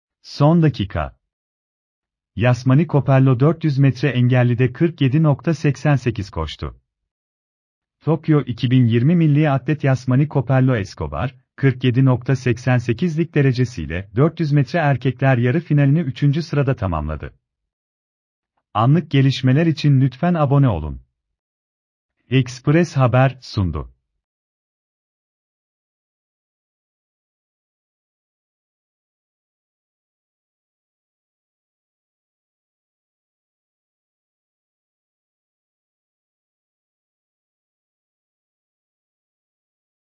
Son dakika... Yasmani Copello 400 metre engellide 47.88 koştu
Tokyo 2020 milli atlet Yasmani Copello Escobar, 47.88’lik derecesiyle 400 metre erkekler yarı finalini üçüncü sırada tamamladı.